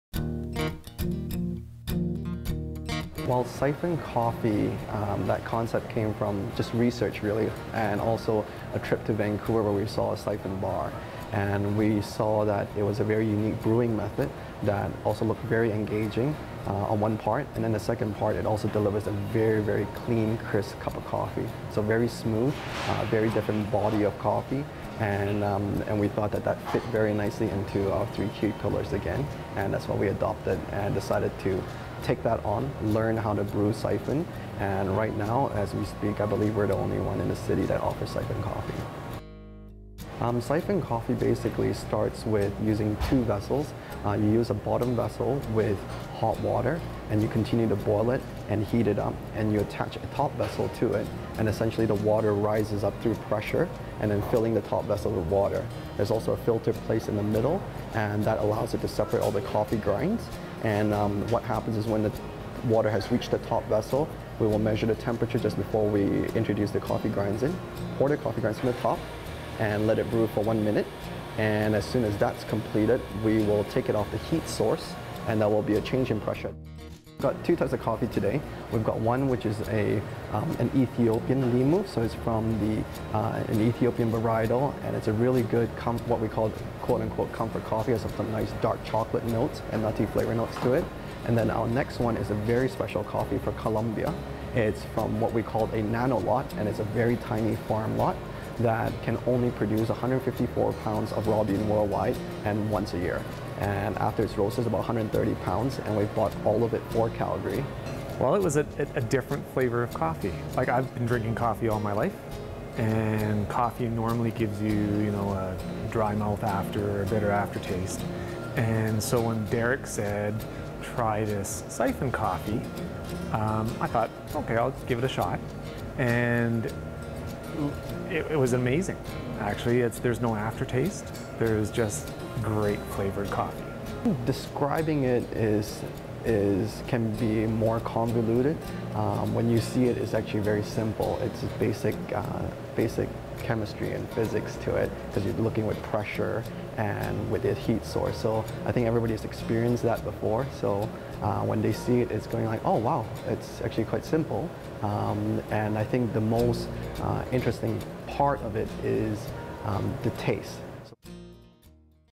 Cafe Blanca
There is a unique way of brewing coffee that is hard to find in Calgary.